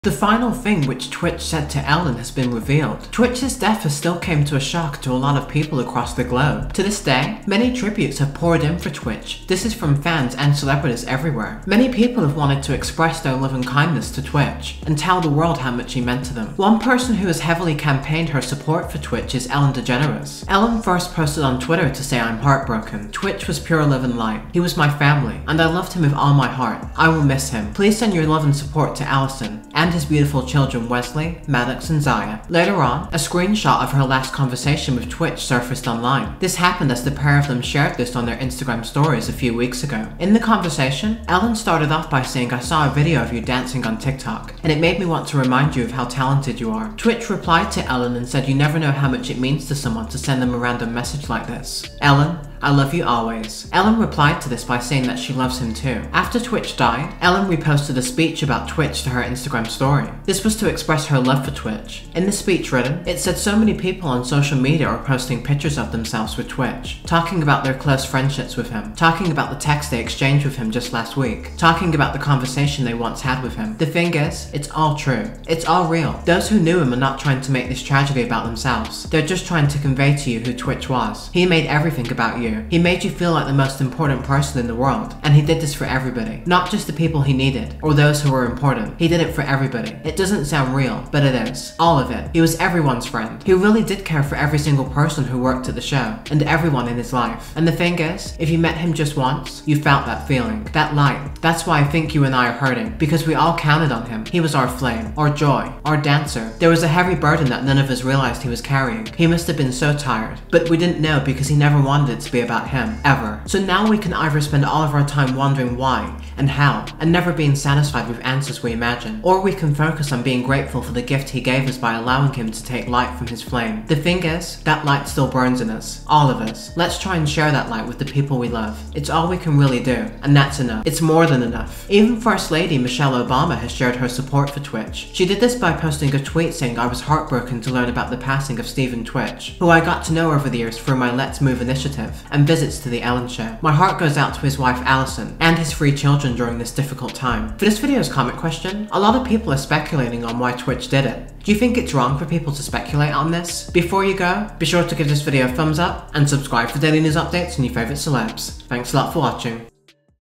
Ellen DeGeneres Reveals The Last Thing tWitch Said To Her Before He Passed Away
The final thing which DJ Stephen tWitch said to Ellen has been revealed!